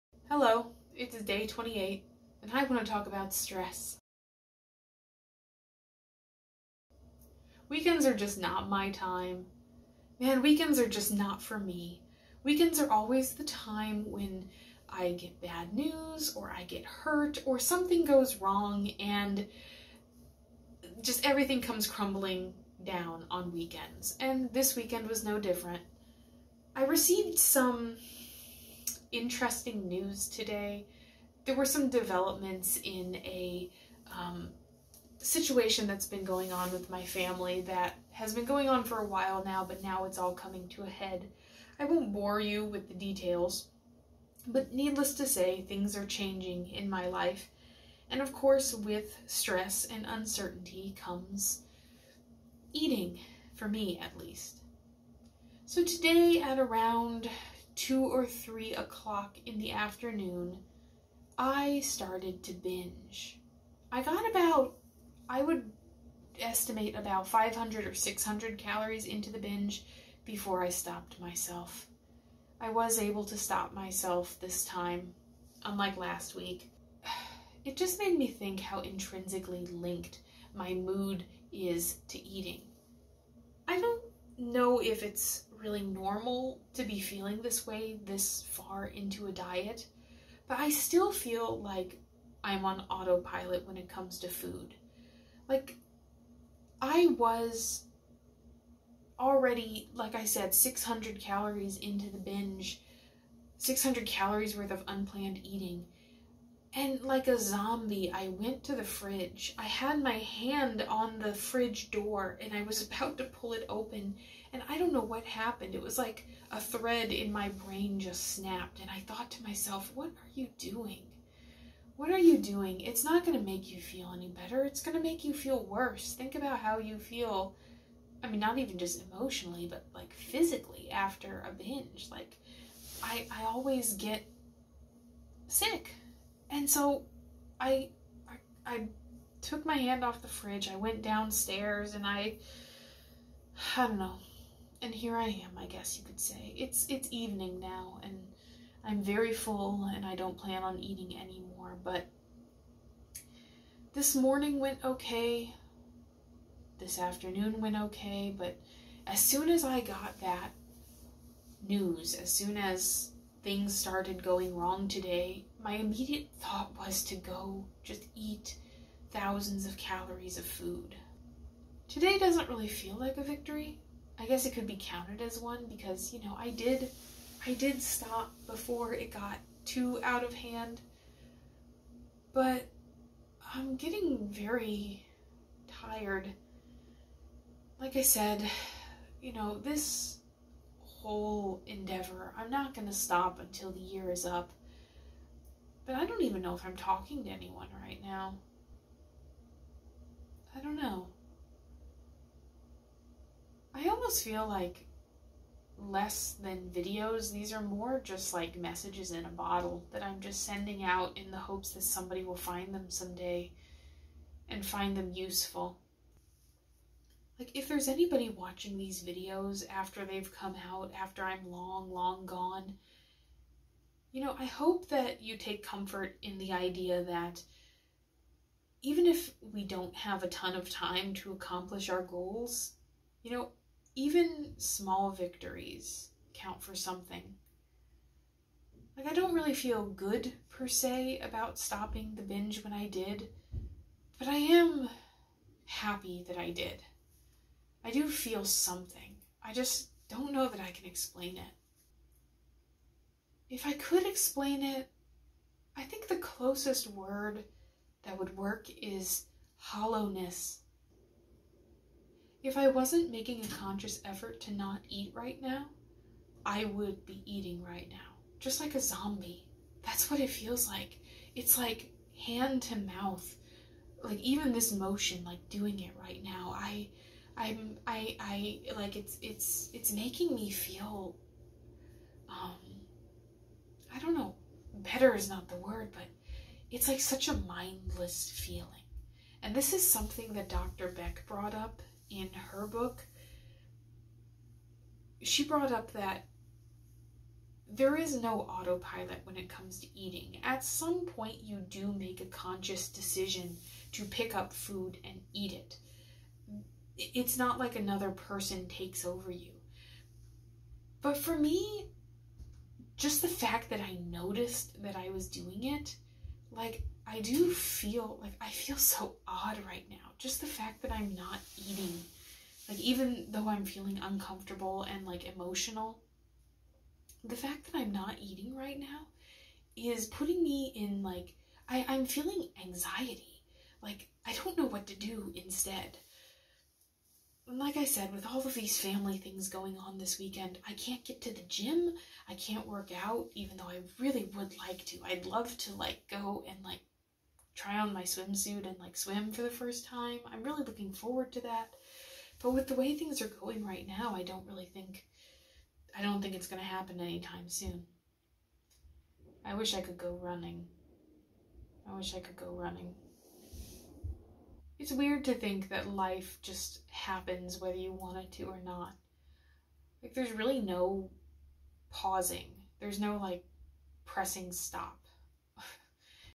Day 28: Stress
Your favorite zombie tries to explain why not overeating is hard.